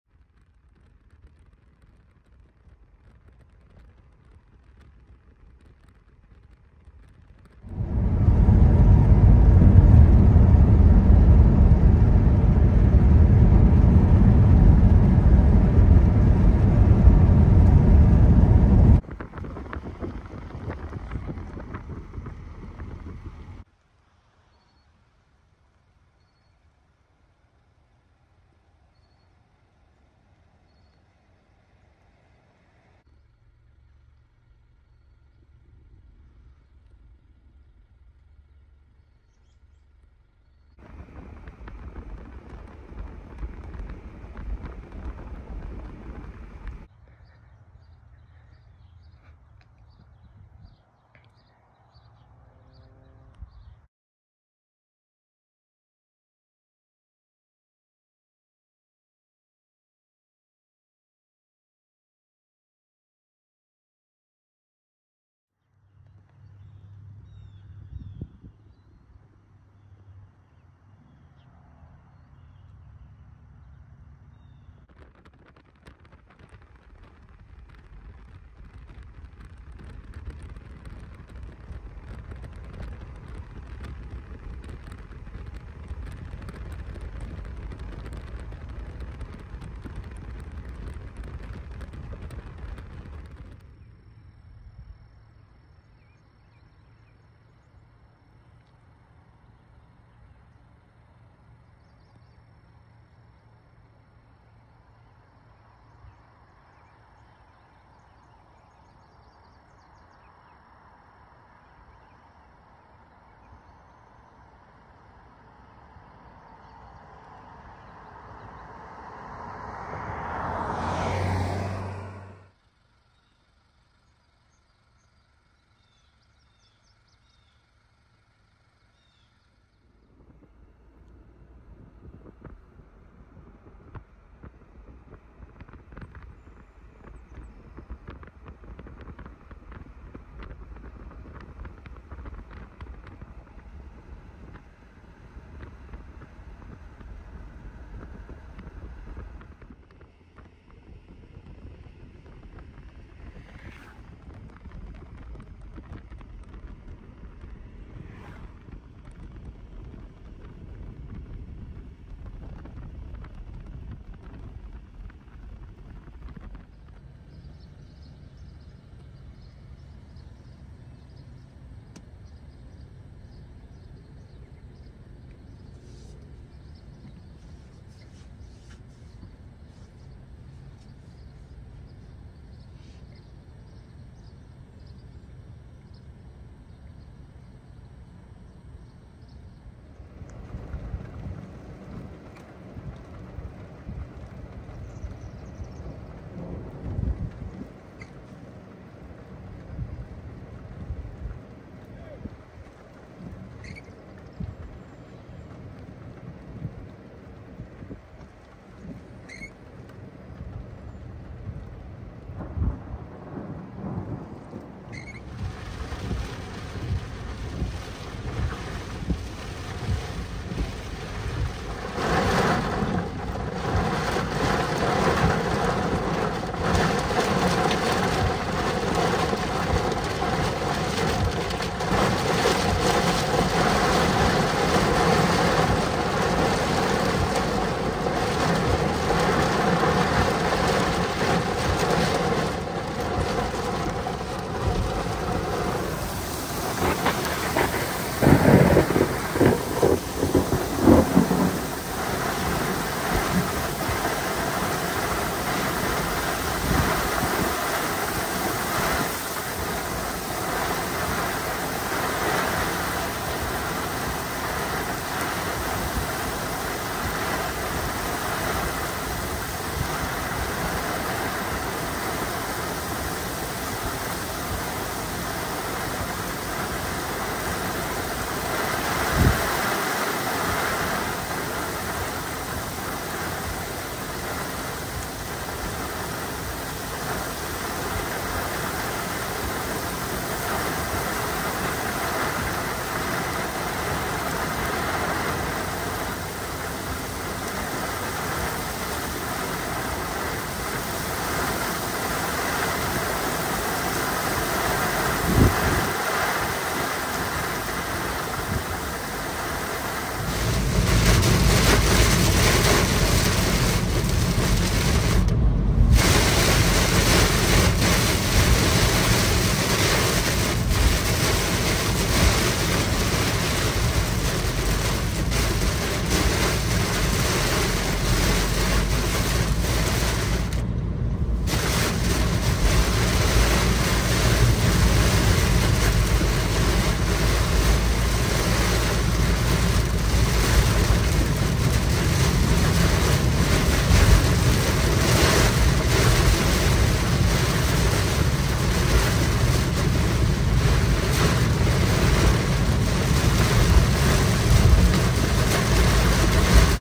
Portage/Mahoning County Ohio Supercell (7-8-23)
Yesterday’s storm chase (7-8-23) was kinda a surprise for me. I was not expecting any storms or even strong ones heck even a supercell. Spc did have areas west of Akron in a general for a gusty storm and east of Akron was a general risk of storms. Anyway I noticed tiny cells off to the west near the Akron area and I was hesitant on taking a look at them since they appeared weak. However a storm chasing friend of mine convinced me to check it out and well, let’s just say I was not expecting what I saw! Chased the storm from Uniontown back toward the Austintown, Ohio area. Anyway, I hope you enjoy this video as this was quite a fun chase for how local it was! ￼